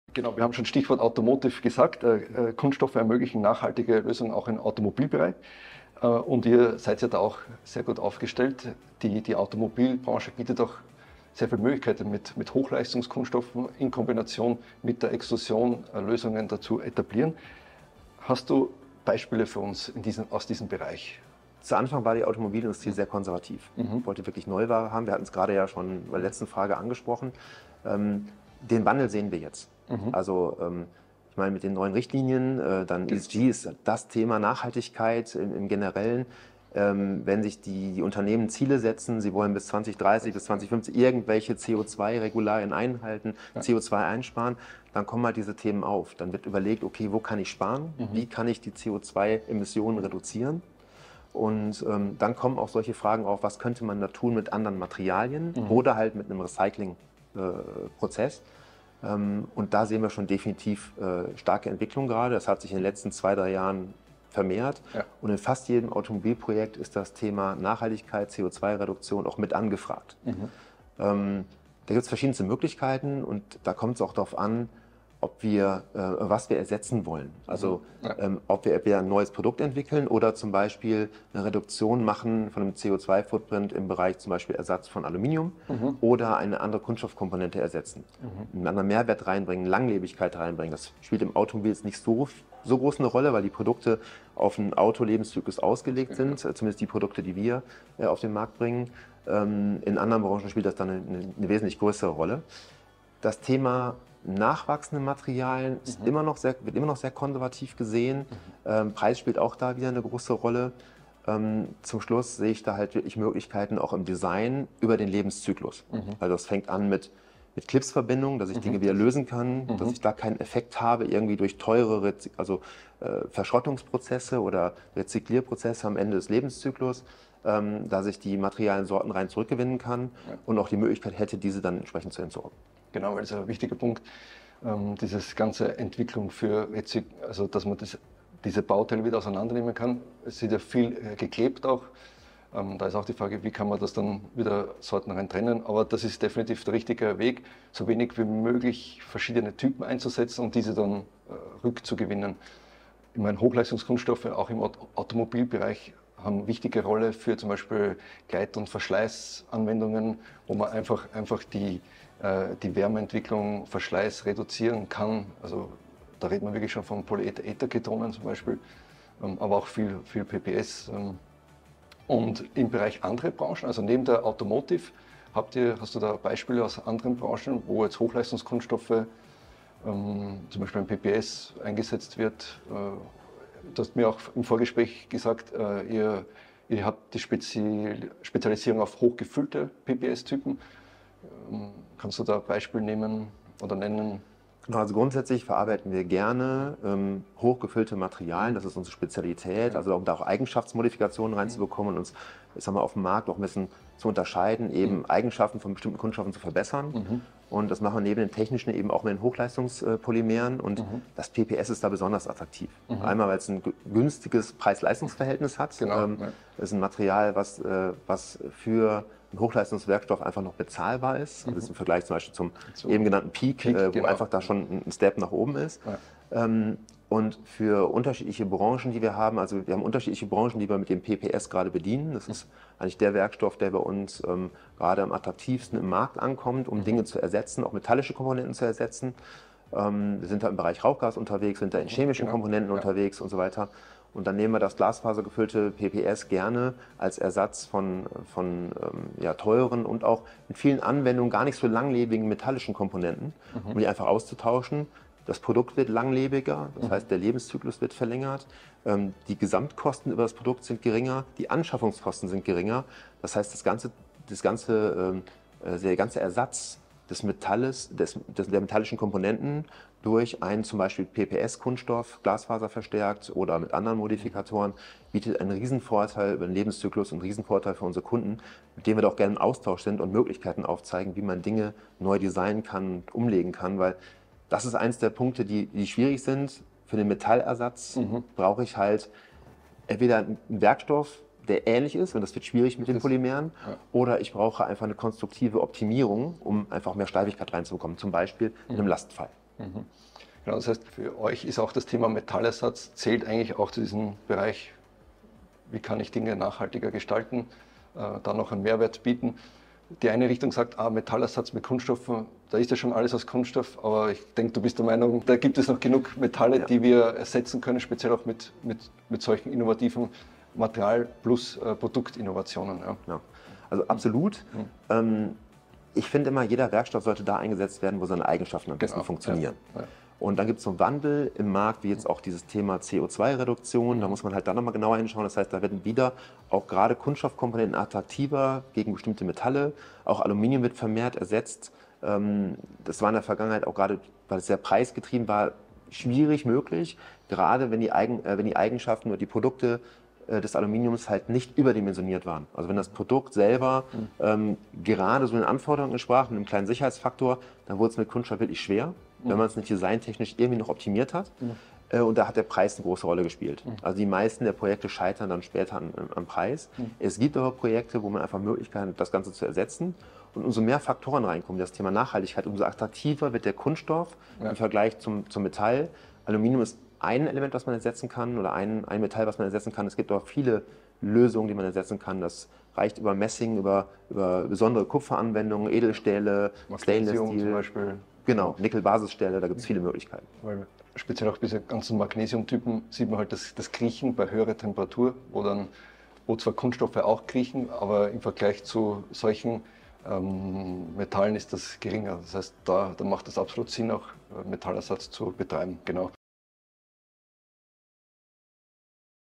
Experteninterview Teil 2: Kunststoff ermöglicht nachhaltige Lösungen für Automotive
Polymer Experte und Autor des Blogs findoutaboutplastics.com Dr. Herwig Juster und Dirk Moses, Head of Market Development bei Technoform, erläutern, wie Kunststoffe in verschiedenen Branchen nachhaltige Lösungen ermöglichen. Die Kunststoffexperten geben uns einen Einblick in die spannende Welt der Innovationspotenziale thermoplastischer Rohstoffe.

Die Automobilbranche bietet ein enormes Potenzial für den Einsatz von Hochleistungskunststoffen im Kontext der Nachhaltigkeit. Dirk erläutert anhand eines Beispiels, wie Kunststoffkomponenten die Optimierung von Batteriepacks für Hochspannungsbatterien ermöglichen und wie Kunststoffe eine effektive Entkopplung von Komponenten ermöglichen.

Aber nicht nur die Automobilbranche profitiert von den Vorteilen der Kunststofftechnologie. Herwig zeigt uns, wie Hochleistungspolymere wie PPS in der Behandlung korrosiver Flüssigkeiten und Gase eingesetzt werden können, um erhebliche Mengen an CO2 einzusparen. Diese Kunststoffe bieten nicht nur eine lange Lebensdauer, sondern auch weitere Vorteile entlang der Wertschöpfungskette, wie geringeres Gewicht und einfachere Montage.

Des Weiteren diskutieren Dirk und Herwig den Ersatz von Metallen durch Kunststoffe. Während Metalle unverzichtbar bleiben, gibt es Anwendungen, in denen Kunststoffe eine effiziente Alternative darstellen können. Beispielsweise können recycelte Kunststoffe Metalle in bestimmten Anwendungen ersetzen und gleichzeitig einen Mehrwert schaffen, indem sie die Vorteile der Kunststofftechnologie nutzen.

Tauchen Sie mit uns ein in die Welt der nachhaltigen Kunststoffinnovationen und entdecken Sie, wie Technoform und andere Unternehmen dazu beitragen, eine umweltfreundlichere Zukunft zu gestalten. Interessante Beispiele und wegweisende Lösungen für eine nachhaltigere Industrie!
Technoform spezialisiert sich auf die Extrusion von Kunststoffprofilen.